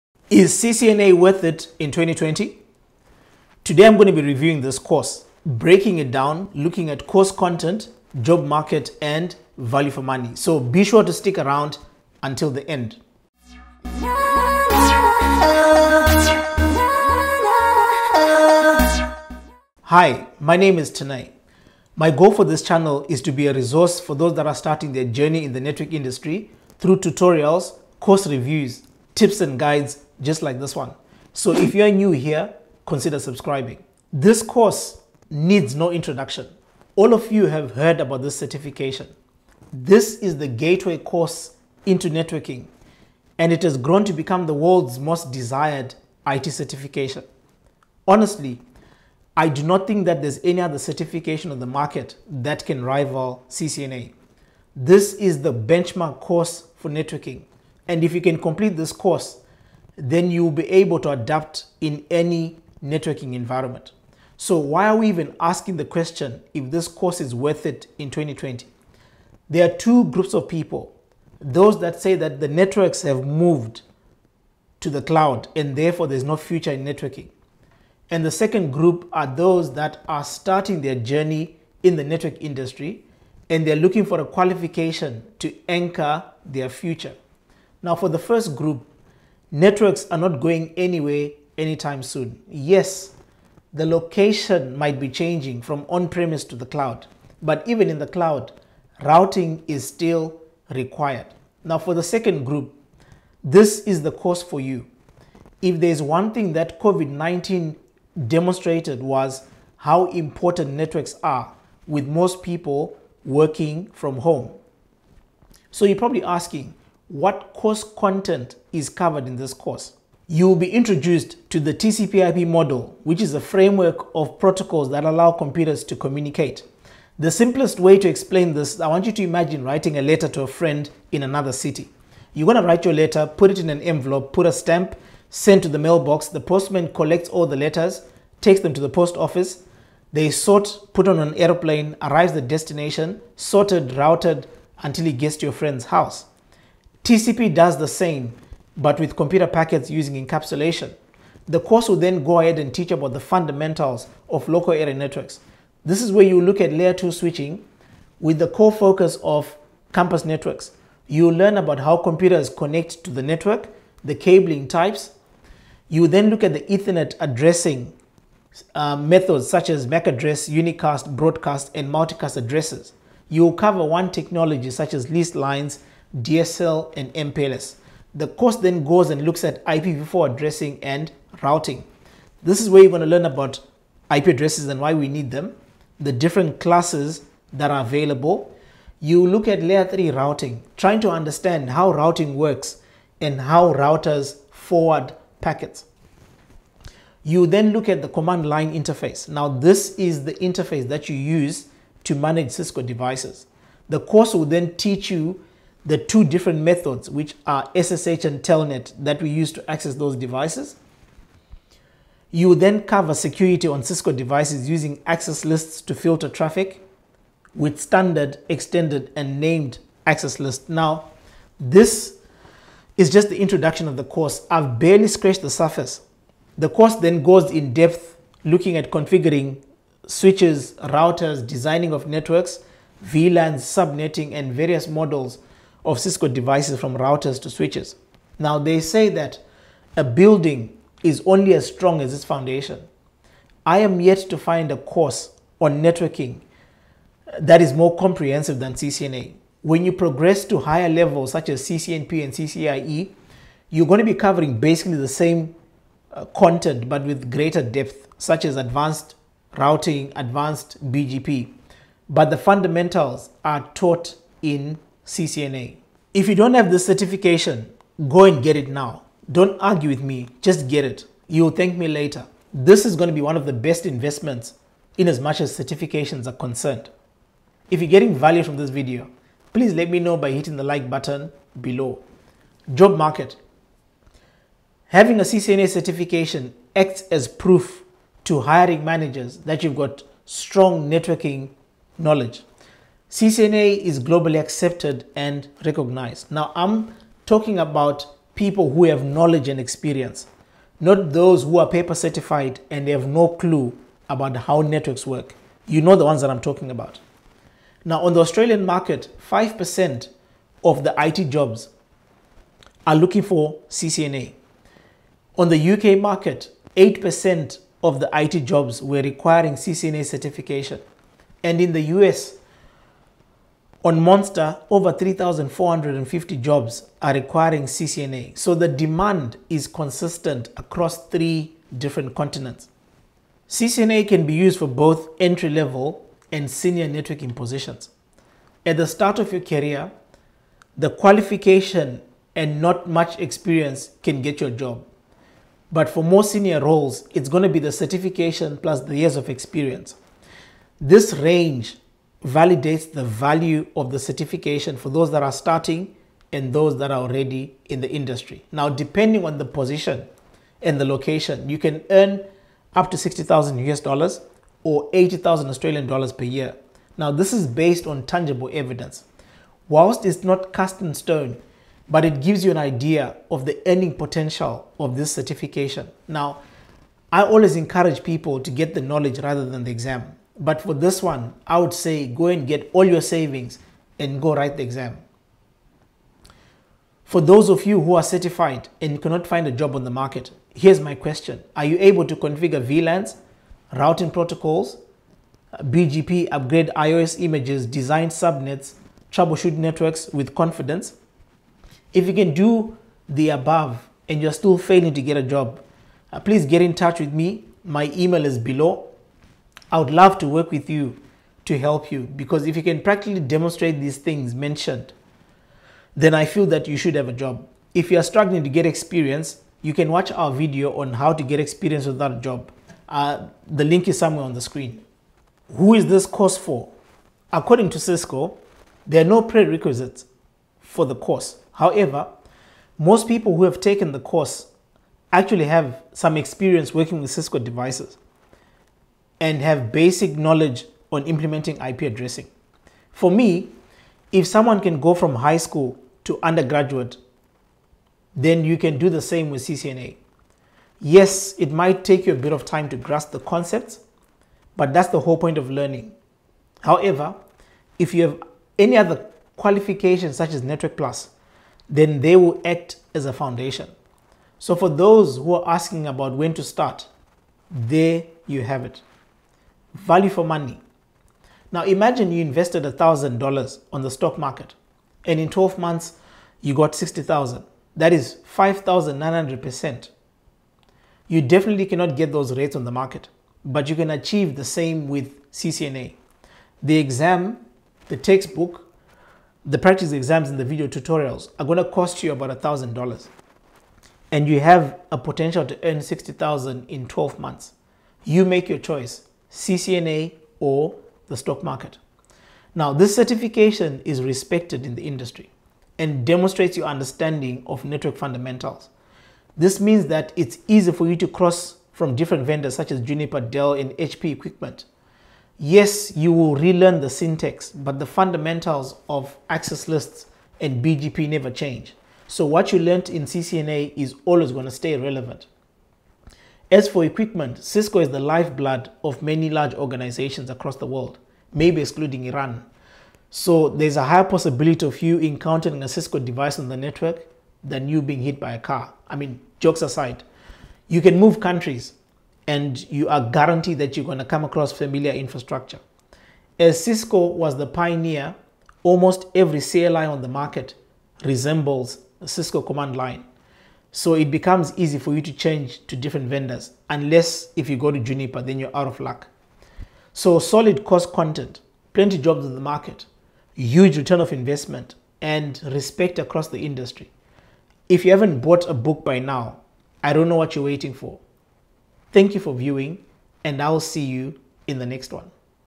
Is Cisco CCNA Worth it in 2020? Don't start your course without watching this video.. (Part 3)
Is Cisco CCNA still worth it in 2020? Can you get a job on the market with your certification? Networks are moving to the cloud, should you go ahead with CCNA? Lets find out..

In this video I will breakdown the course looking at course content, job market, and value for money to help you make that decision.

Timeline
0:00 - Intro
1:54 - Course Content
2:31 - Fundamentals of LAN
2:56 - IPv4 Addressing and Routing
4:46 - Job Market
7:04 - Who is this course for?
7:51 - Value for Money

Study Material